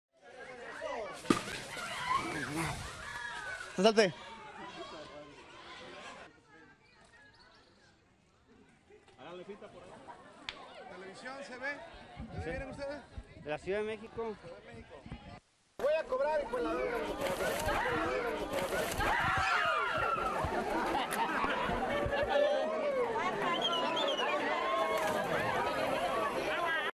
تمساح يفاجئ سكان حي بالمكسيك
تشهد المكسيك عواصف عاتية وفيضانات بسبب الإعصار إنغريد، إلا أن المفاجأة كانت بانتظار سكان أحد الأحياء الذين فوجئوا بتمساح جرفته المياه إلى حيهم. لمشاهدة المزيد، يرجى زيارة الموقع الإلكتروني لسكاي نيوز عربية من خلال الضغط على الرابط التالي